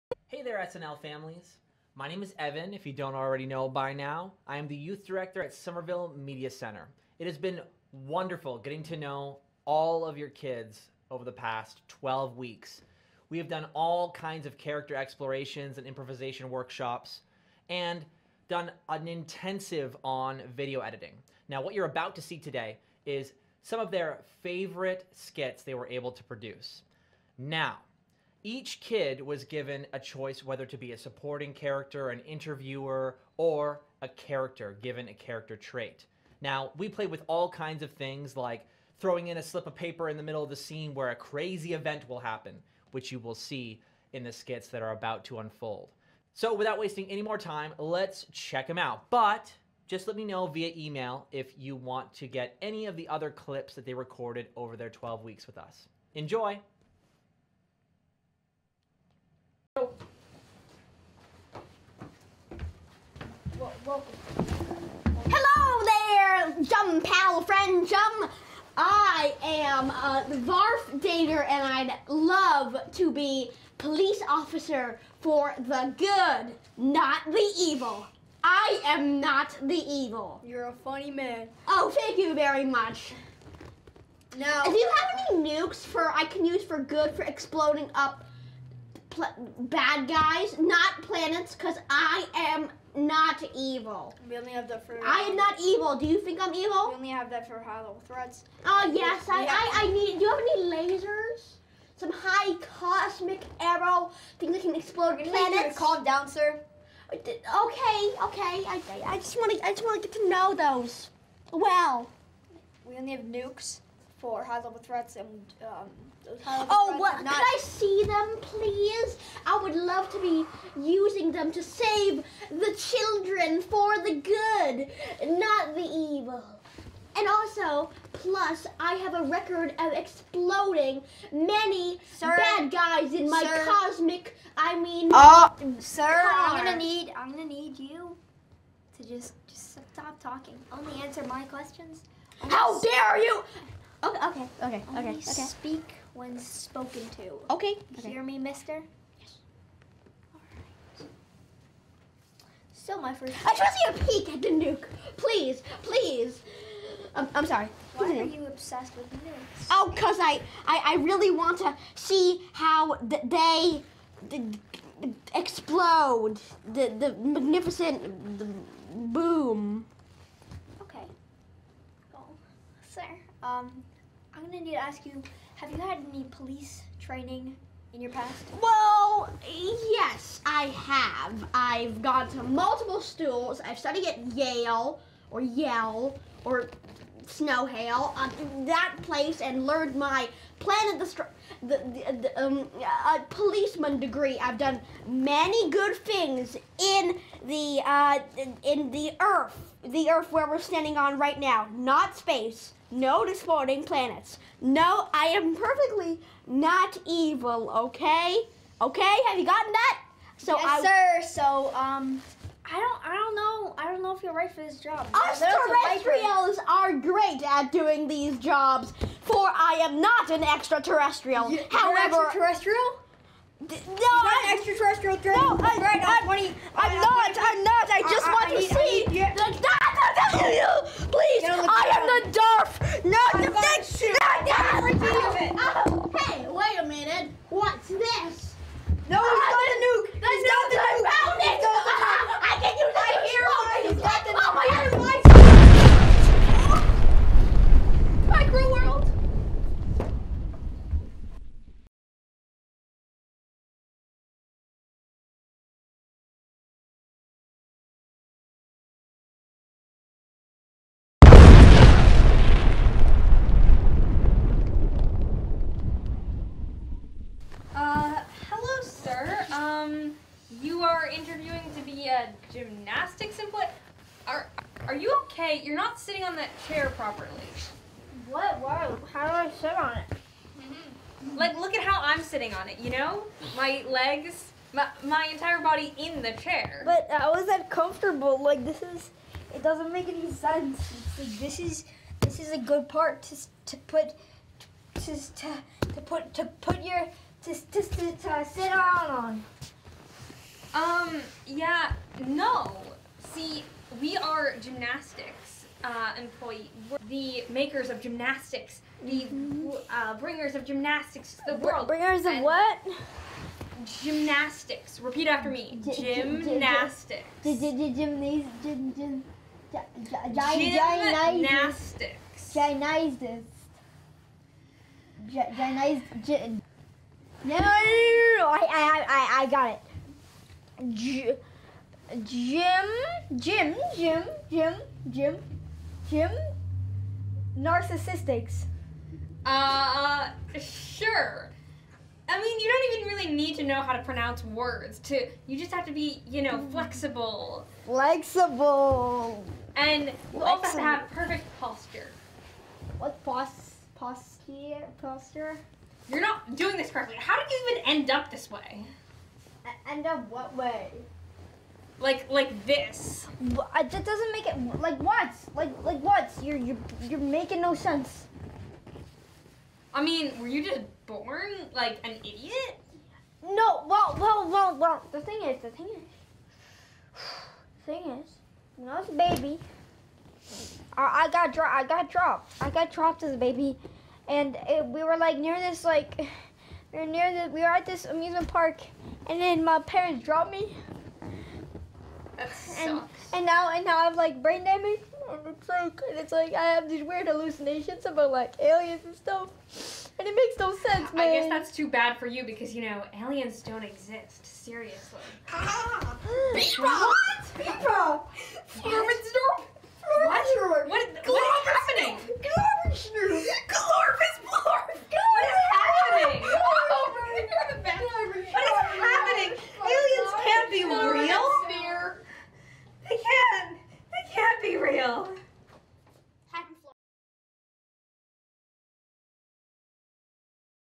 Somerville Night Live Fall 2021 Showcase
Take a look at the hilarious skits our students made this semester! Happy Holidays everyone!